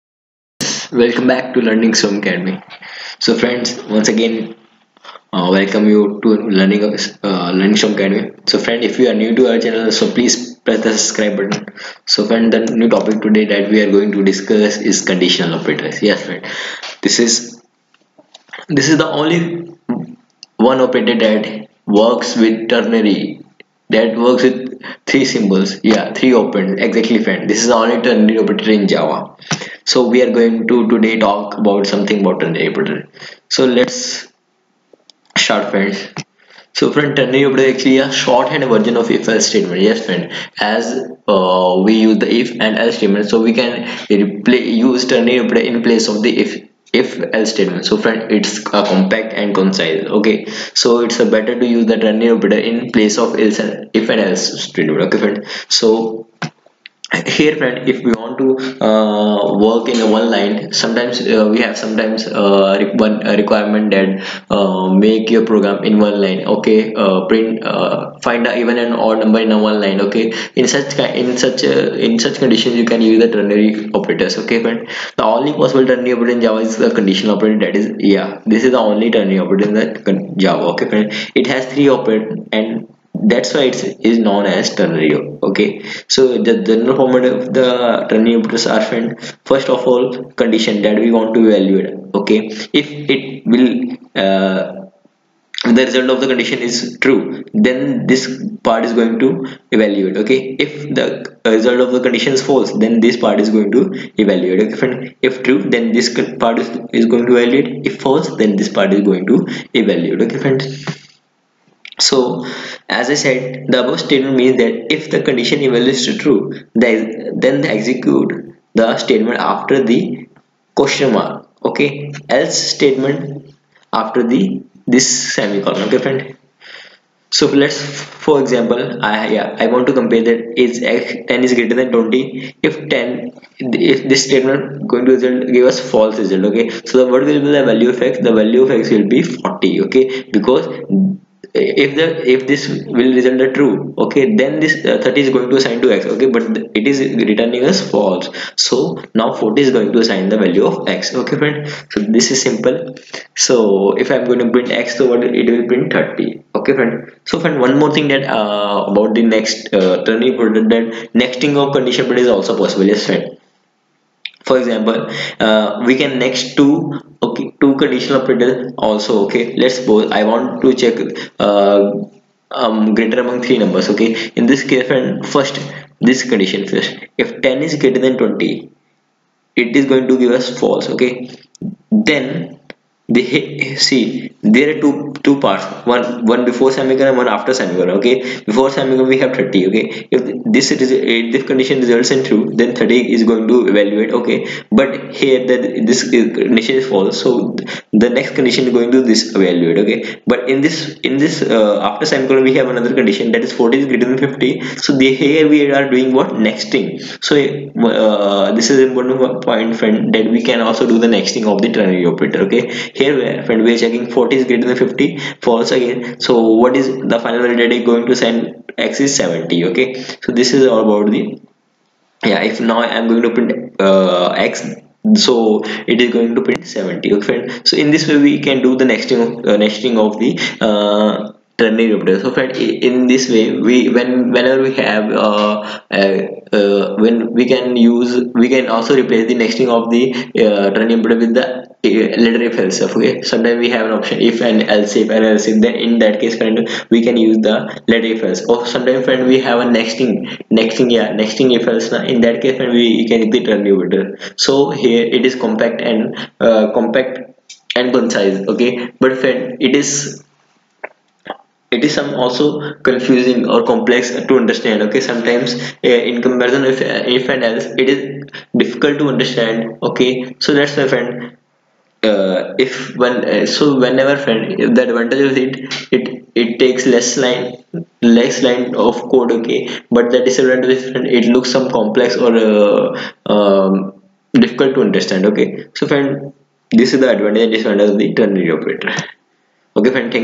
Lecture 28:- Conditional operator in java|| LearningStrome Academy
# conditional operator in java
# Is nesting of conditional operator is applicable or not?
# Advantage of conditional operator in java.
# disadvantage of conditional operator in java.